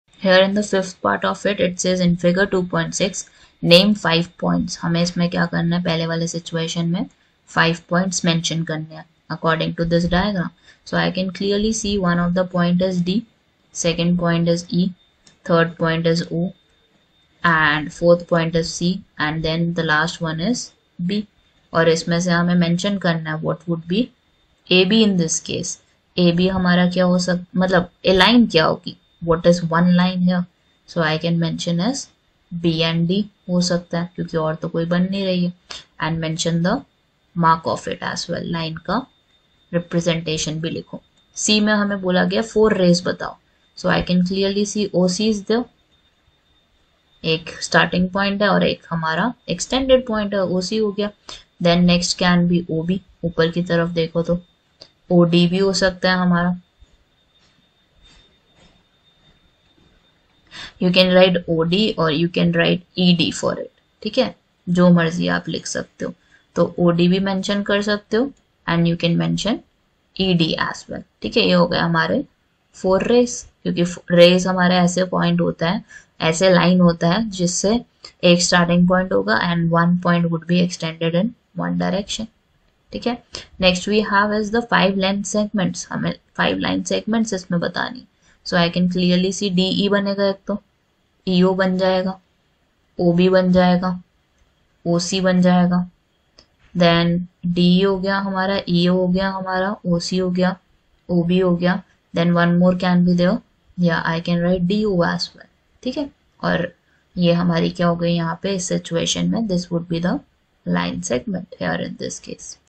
5. In Fig. 2.6, name: a. Five points b. A line c. Four rays d. Five line segments
5. In Fig. 2.6, name:
a. Five points
 b. A line
 c. Four rays
 d. Five line segments